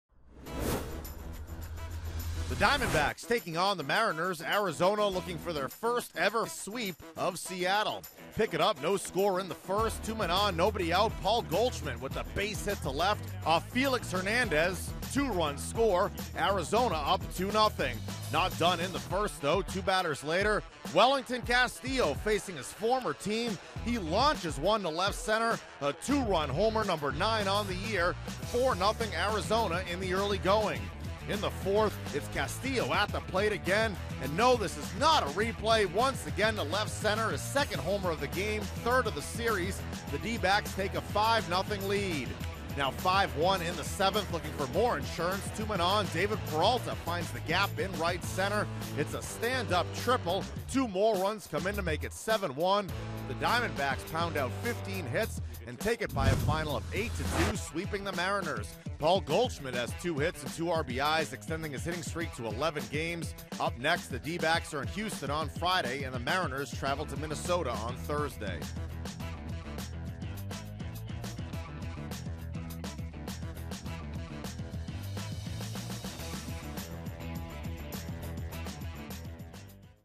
7/29/15: Corbin, D-backs break out brooms in Seattle
Daily Recap: Patrick Corbin allowed one run over six frames and Welington Castillo hit two home runs as the D-backs completed the sweep